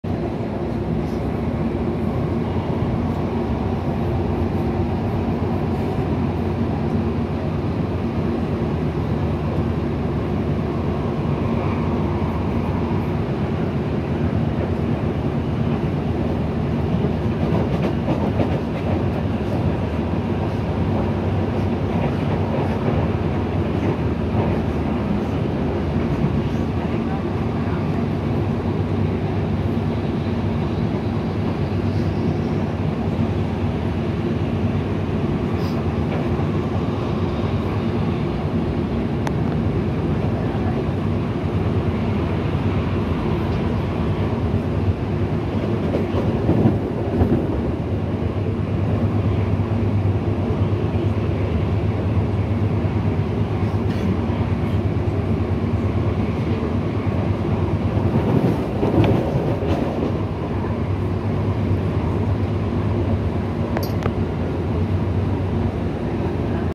Metro de medellin
Trenes a mas de 80 km/h sobrepasando vehiculos en la av regional.